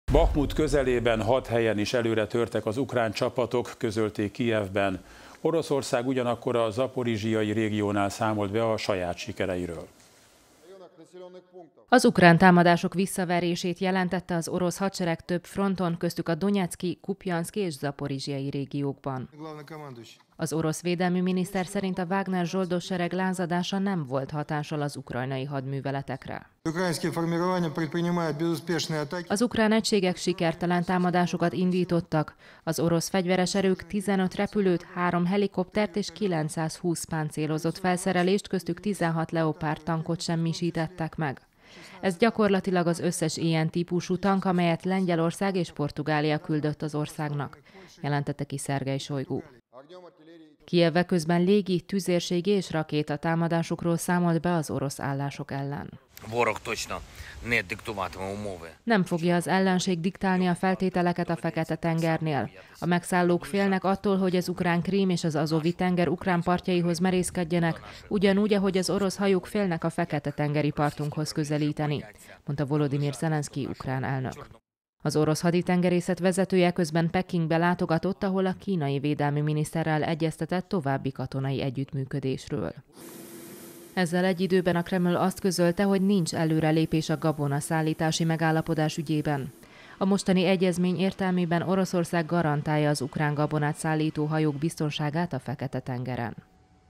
Saját sikereikről számolnak be az orosz és az ukrán erők is
Bahmut közelében hat helyen is előretörtek az ukrán csapatok - közölték Kijevben. Oroszország ugyanakkor a zaporizzsijai régiónál számolt be a saját sikereiről.